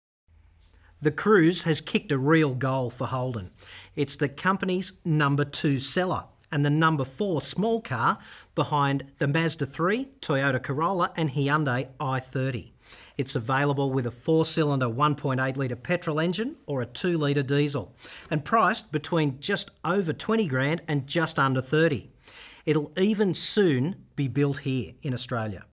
Holden Cruze Review
Holden's no.2 seller, so is it right for you too?